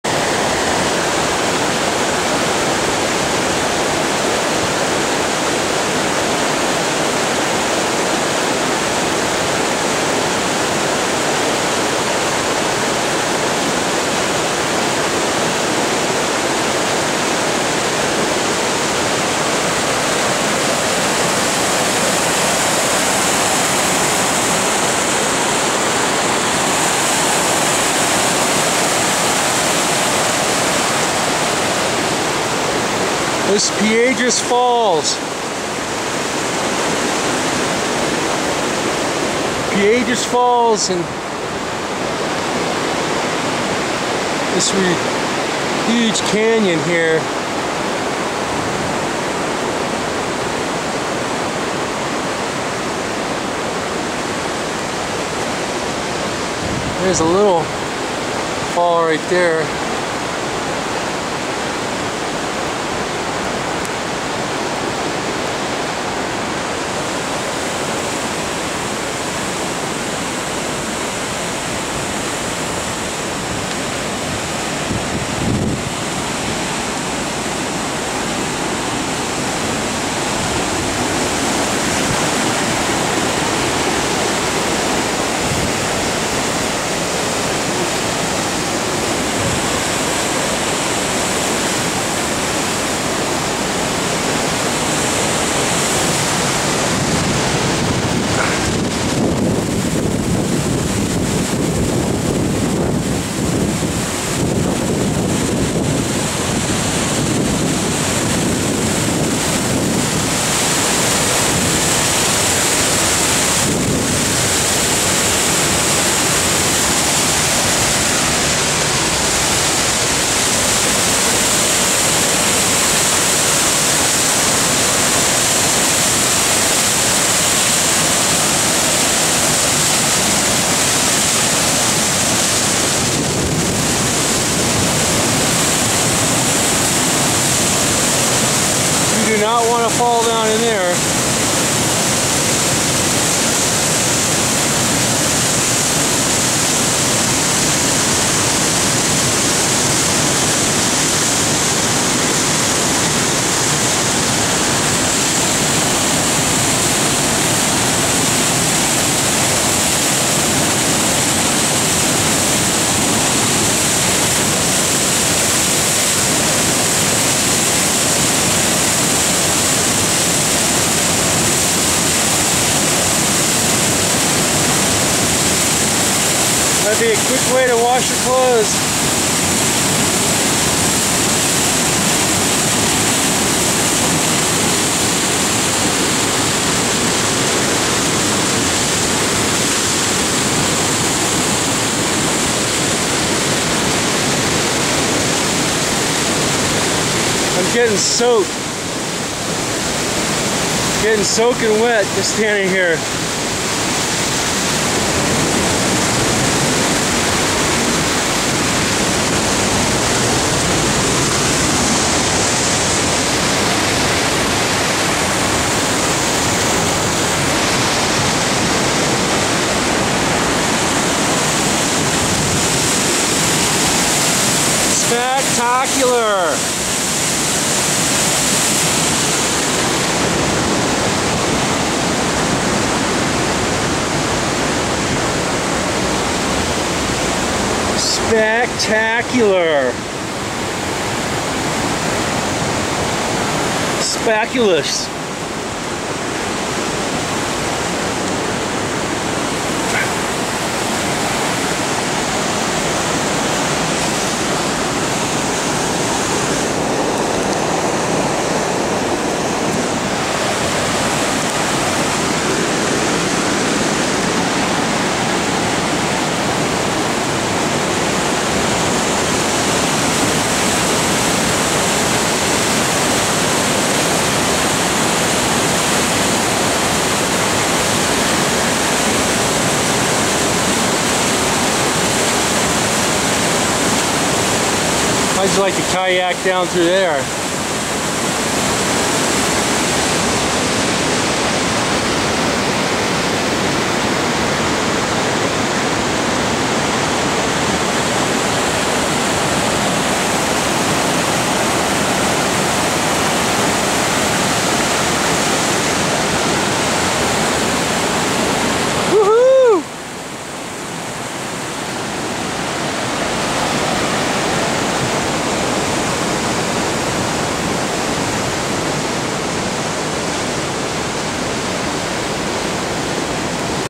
Piedras Falls May 2011
Amazing Spring waterfall in Piedras Canyon north of Pagosa Springs on Mothers Day 2011.